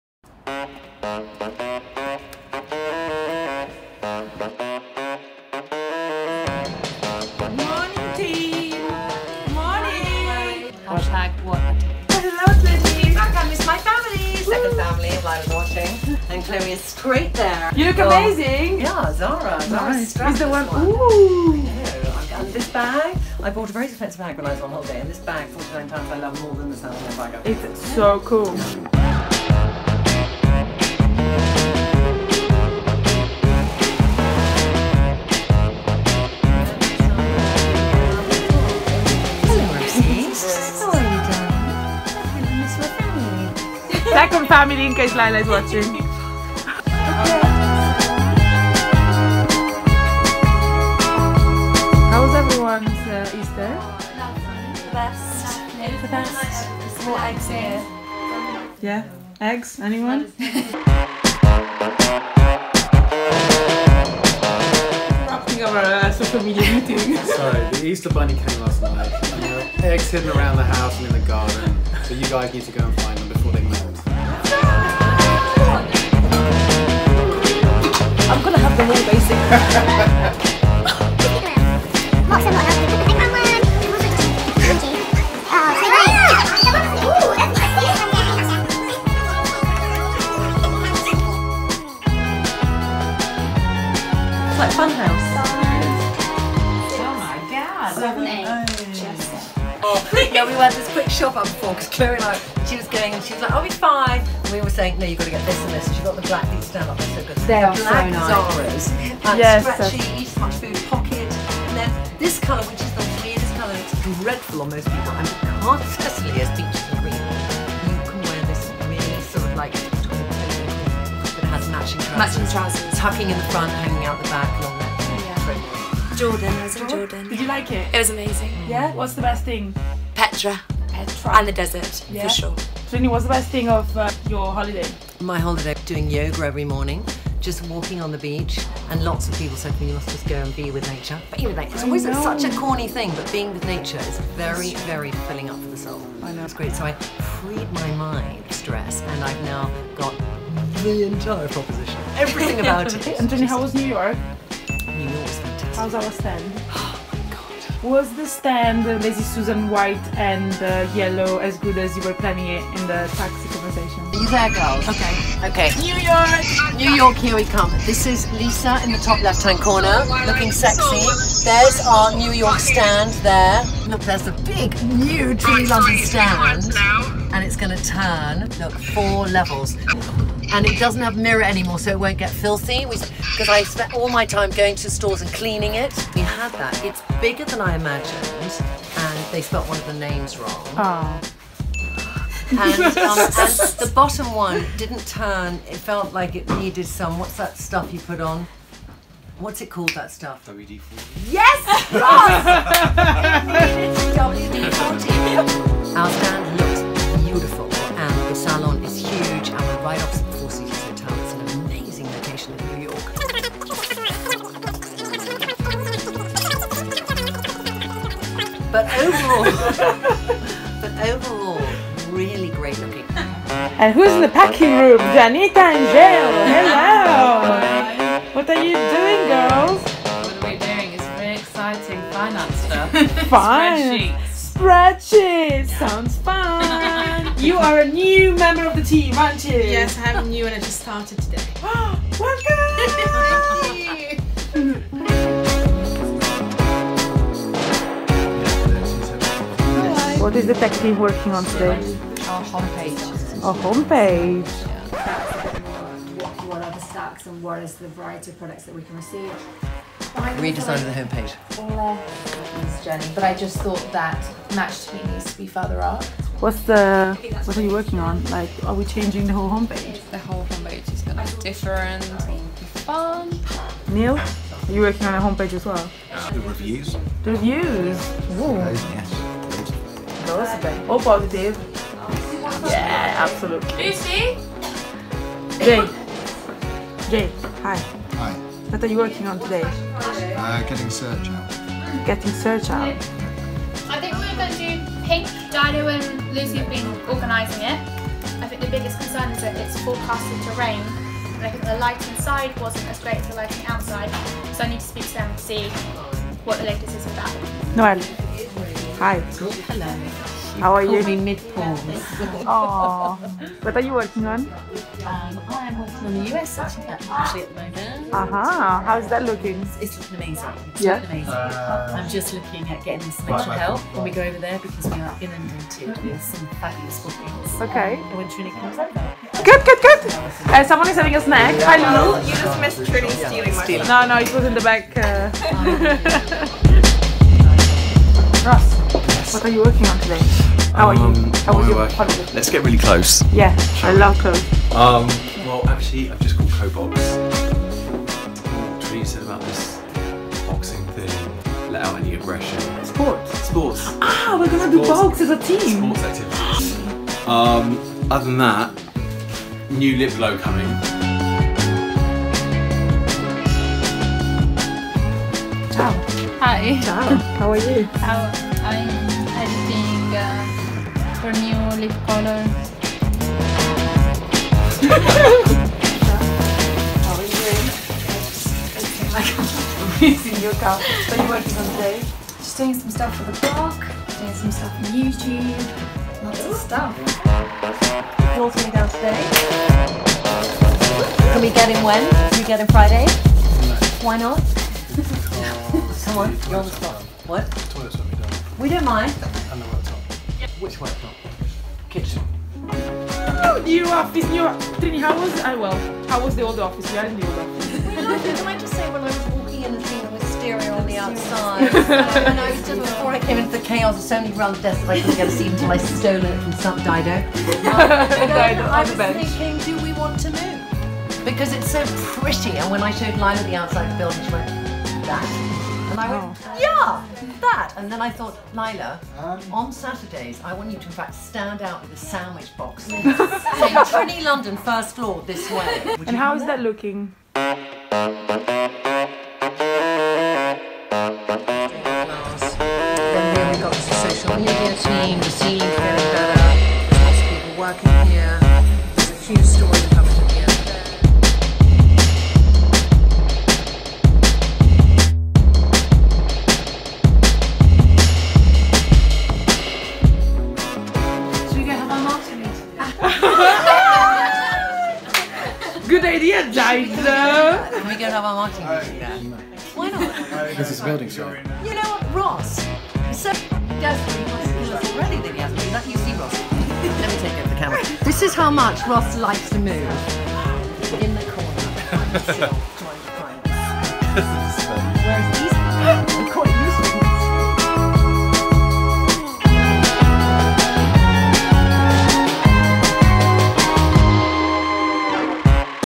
What The T? | EP01 | Trinny
Want to know what happens behind the scenes at TRINNY LONDON HQ? Meet the faces you see on the feed, and see how we’re building London’s hottest new makeup brand...

This week, back from Easter break, Trinny Woodall sparks excitement about our new office…but will we move in on time?! The team plan for our latest product launch and catch up on all the goss about how things are heating up in New York 🗽

For more of What the T, go to the T-TIME Blog